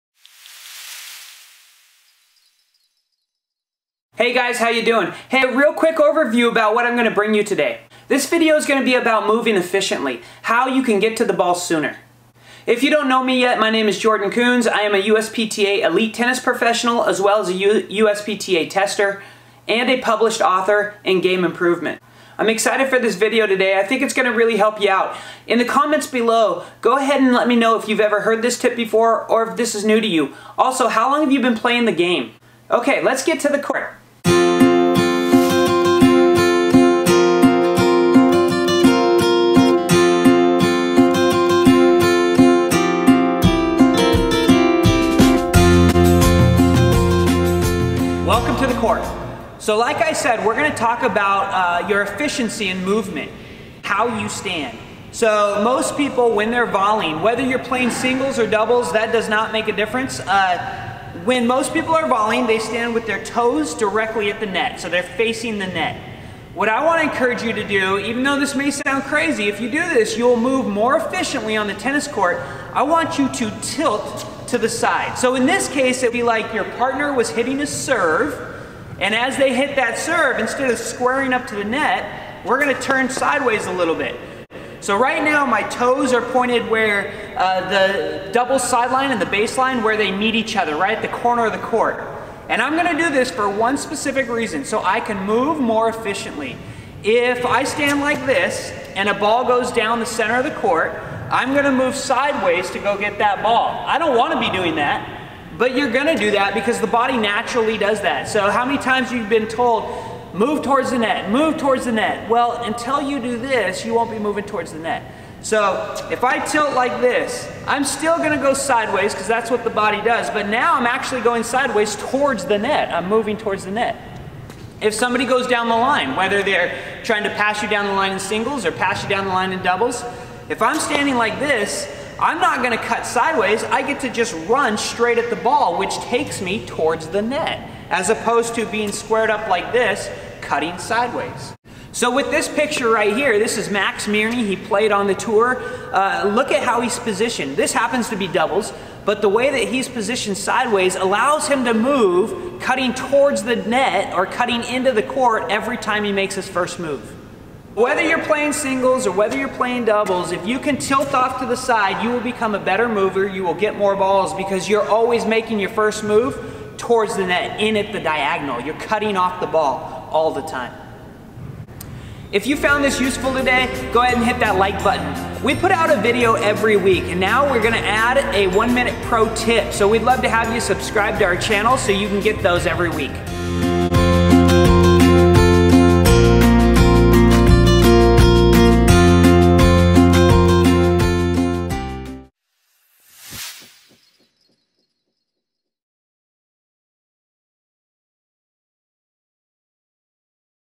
How to Cut Off The Angles / Tennis Beyond Tennis Channel
Get To The Ball Quicker - How to Cut off the Angle in Tennis. Tennis Beyond Tennis Channel

Efficient Movement in Tennis - Tennis Beyond Tennis Channel
Understanding how Efficient Positioning will allow you to cut off the Angle of the ball. “Examples Roger Federer and Max Miryni
Thanks for looking at our Tennis Channel

Previous episodes